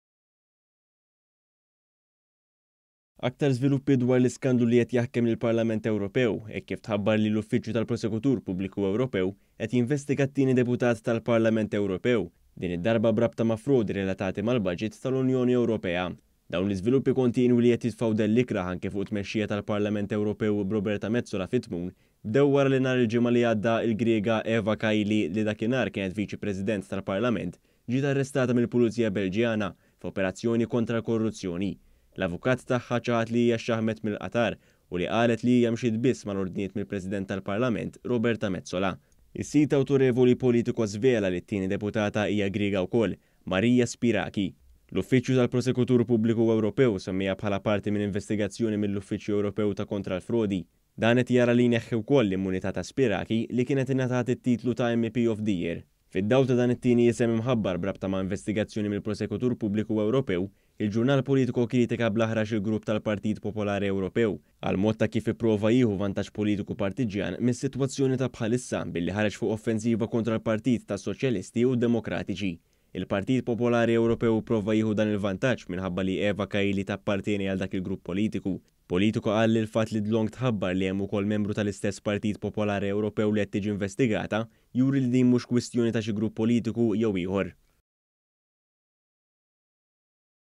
QED TIĠI INVESTIGATA DEPUTATA OĦRA TAL-PARLAMENT EWROPEW
ONE.COM.MT • Aktar żviluppi dwar l-iskandlu li qed jaħkem lill-Parlament Ewropew, hekk kif tħabbar li l-Uffiċċju tal-Prosekutur Pubbliku Ewropew (l-EPPO) qed jinvestiga t-tieni deputat tal-Parlament Ewropew, din id-darba b’rabta ma’ frodi relatati mal-Baġit tal-Unjoni Ewropea.

Dawn l-iżviluppi kontinwi li qed jitfgħu dell ikrah anki fuq it-tmexxija tal-Parlament Ewropew, b’Roberta Metsola fit-tmun, bdew wara li nhar il-ġimgħa li għadda l-Griega Eva Kaili, li dakinhar kienet Viċi President tal-Parlament, ġiet arrestata mill-Pulizija Belġjana f’operazzjoni kontra l-korruzzjoni.  L-avukat tagħha ċaħad li hija xxaħħmet mill-Qatar u qal li hija mxiet biss mal-ordnijiet mill-President tal-Parlament, Roberta Metsola.

Is-sit awtorevoli Politico żvela li t-tieni deputata hija Griega wkoll: Maria Spyraki. L-Uffiċċju tal-Prosekutur Pubbliku Ewropew semmiha bħala parti minn investigazzjoni mill-Uffiċċju Ewropew ta' Kontra l-Frodi (l-OLAF). Dan qed jara li jneħħi wkoll l-immunità ta’ Spyraki, li kienet ngħatat it-titlu ta’ ‘MEP of the Year’.

Fid-dawl ta’ dan it-tieni isem imħabbar b’rabta ma’ investigazzjoni mill-Prosekutur Pubbliku Ewropew, il-ġurnal Politico kkritika bl-aħrax lill-Grupp tal-Partit Popolari Ewropew għall-mod ta’ kif ipprova jieħu vantaġġ politiku partiġġjan mis-sitwazzjoni ta’ bħalissa billi ħareġ fuq offensiva kontra l-Partit tas-Soċjalisti u d-Demokratiċi.

Il-Partit tal-Popolari Ewropew ipprova jieħu dan il-vantaġġ minħabba li Eva Kaili tappartjeni għal dak il-grupp politiku. Il-Politico qal li l-fatt li dlonk tħabbar li hemm ukoll membru tal-istess Partit Popolari Ewropew li qed tiġi investigata juri li din mhux kwistjoni ta’ xi grupp politiku jew ieħor.